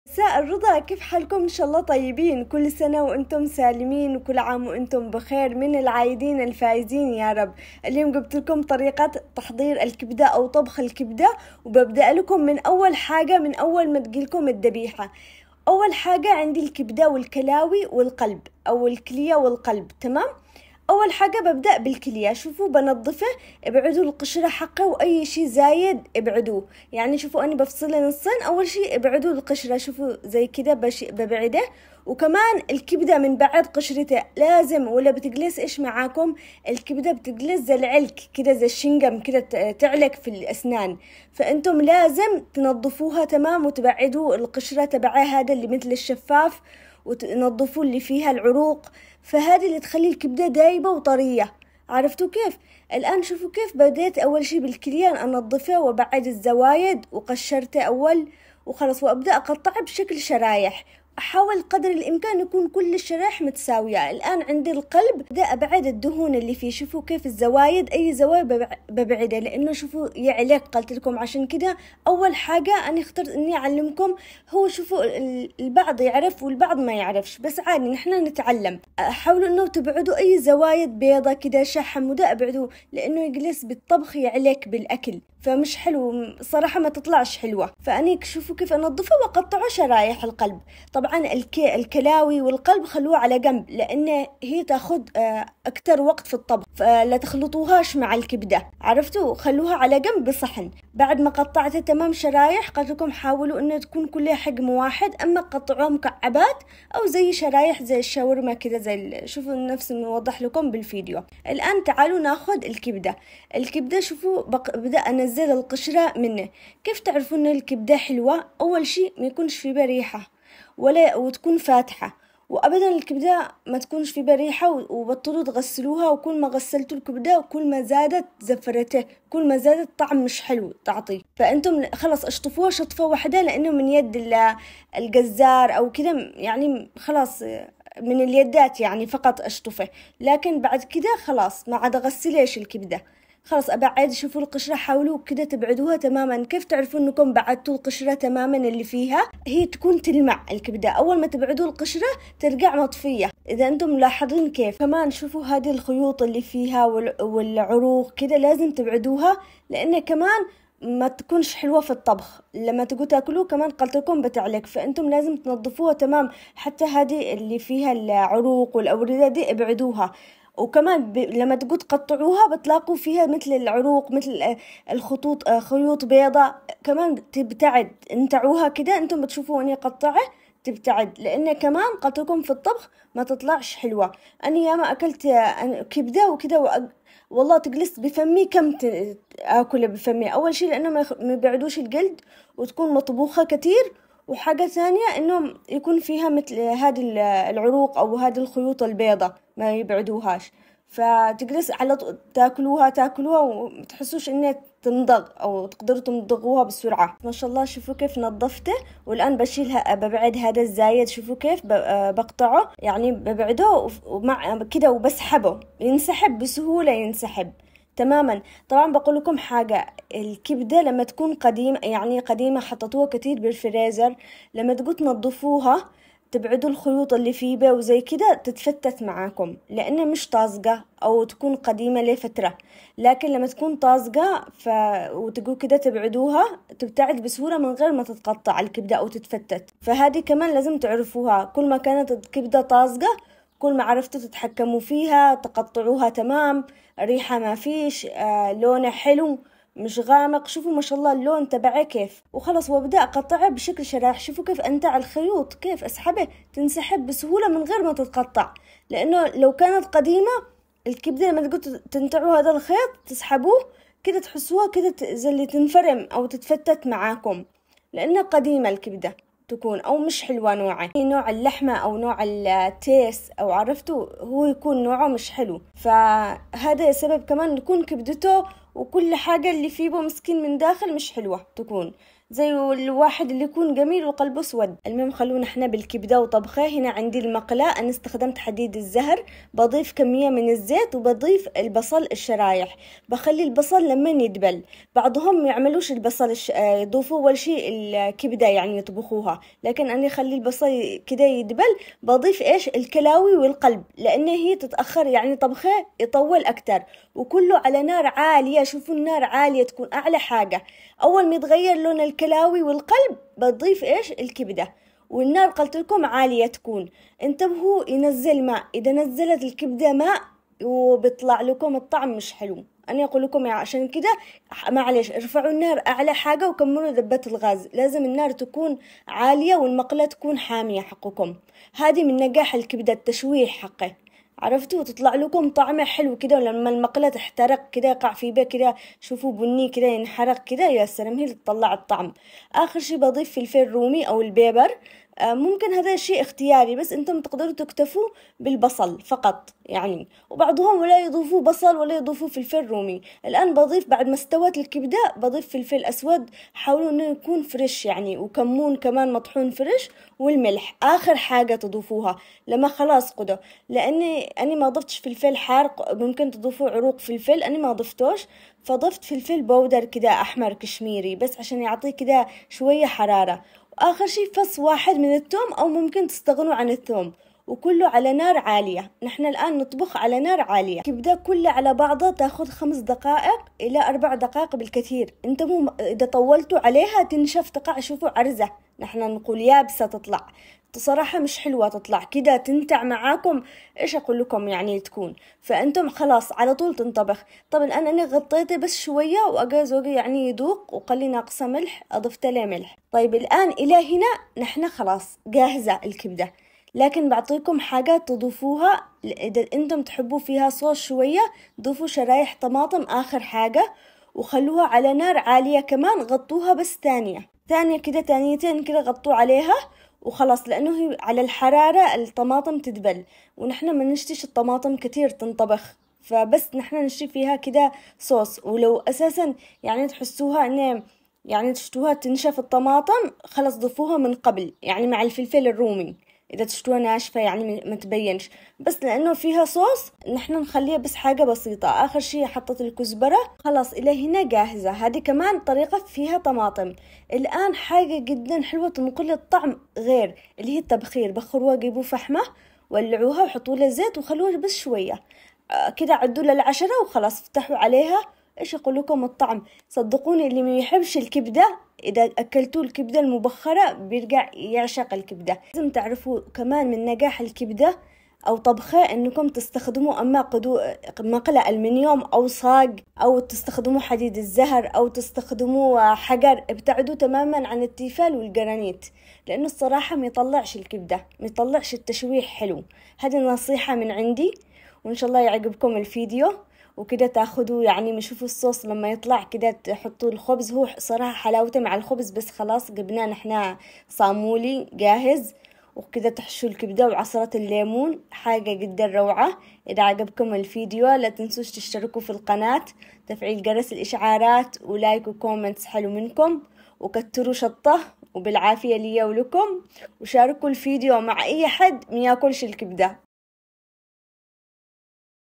الكبدة والكلاوي والقلب طريقة تفصيلية تنظيفها وتقطيعها وطبخها بالطريقة اليمنية
طريقة عمل الكبدة
الكبدة بالطريقة اليمنية
كبدة العيد
تنظيف وتقطيع الكبدة
مكونات الكبدة
كبدة كلاوي قلب
ربع كوب زيت نباتي او قطع من الية الشحم حسب الرغبة
2 حبات بصل مقطع شرائح
فص ثوم او الاستغناء عنه
2 حبه فلفل رومي شرائح
2 حبة طماطم شرائح
رشة فلفل حار او عروق فلفل
ملح حسب الرغبة
كزبراء خضراء
وفحم وزيت للتبخير الكبدة